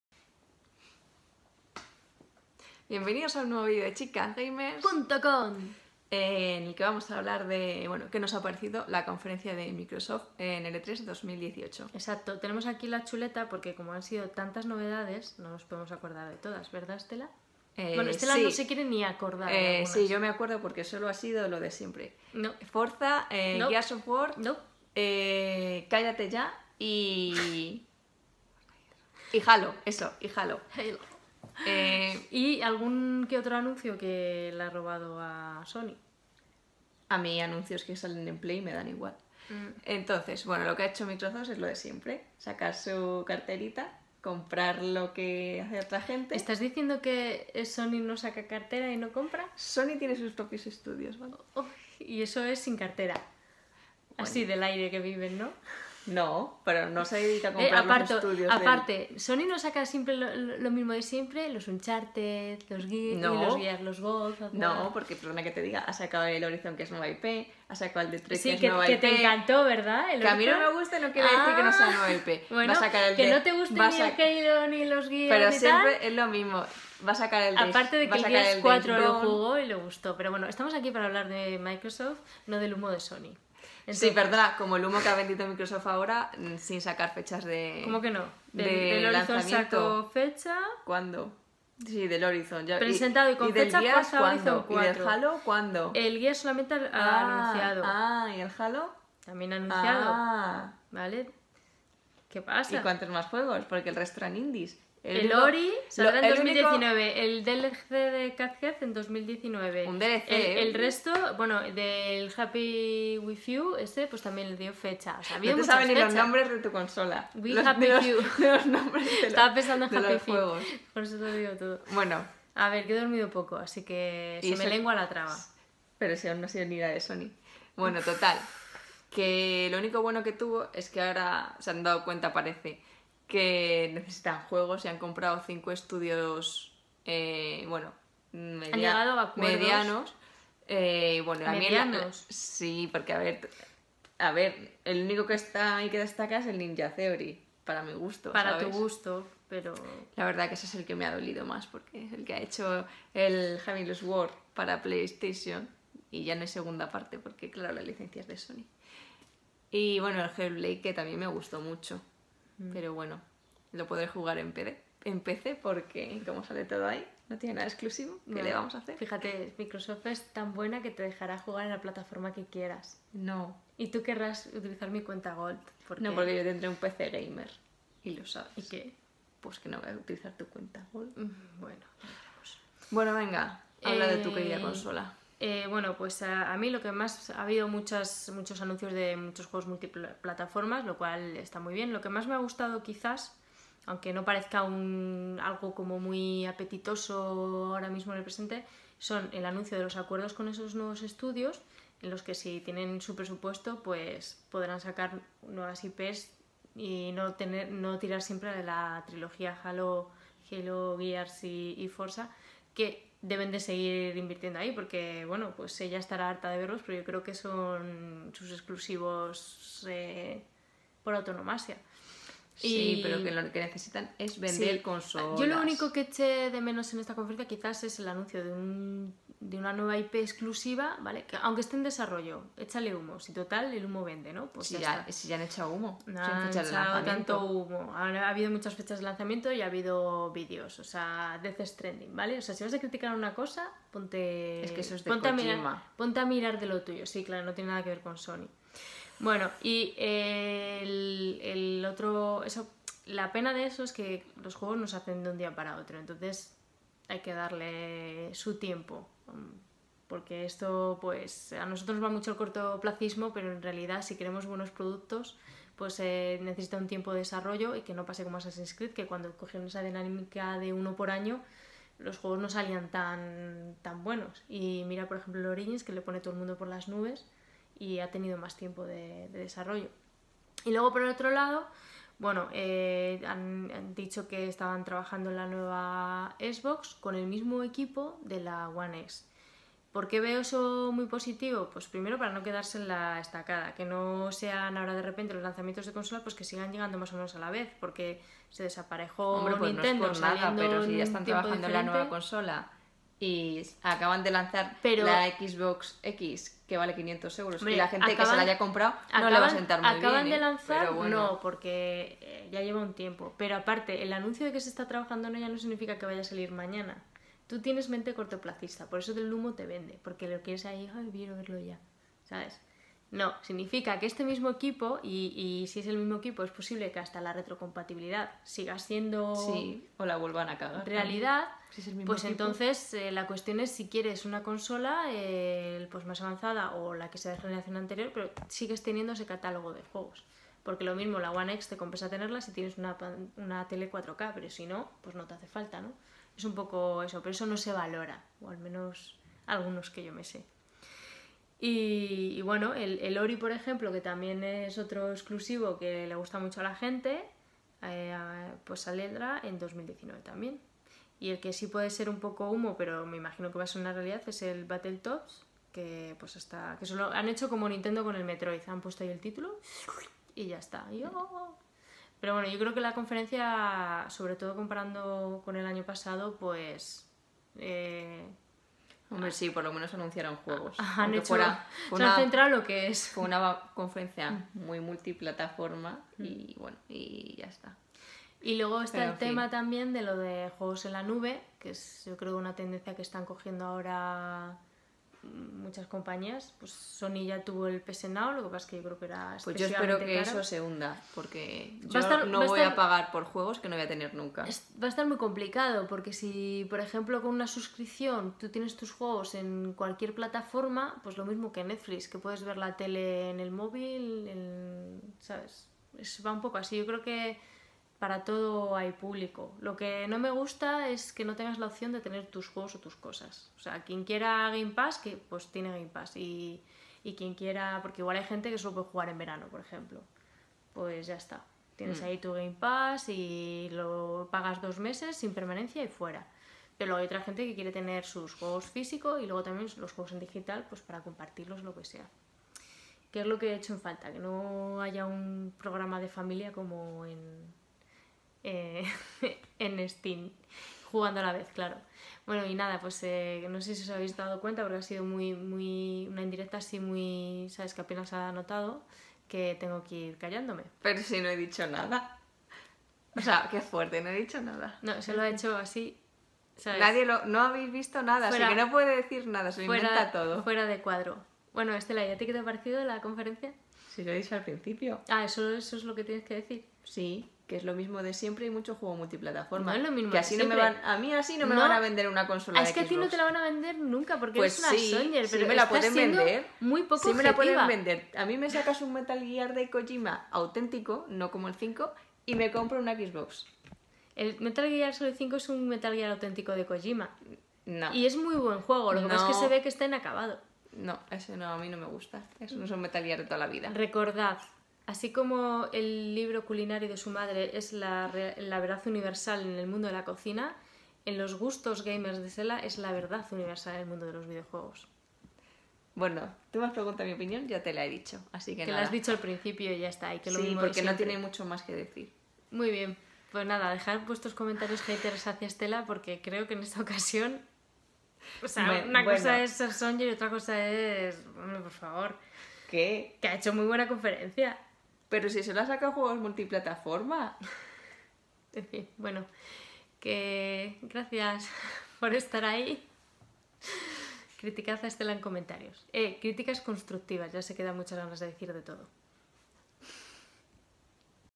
E3 2018 - OPINIÓN CONFERENCIA MICROSOFT
Detalles, anuncios y novedades de la conferencia de Microsoft y Xbox en el E3 2018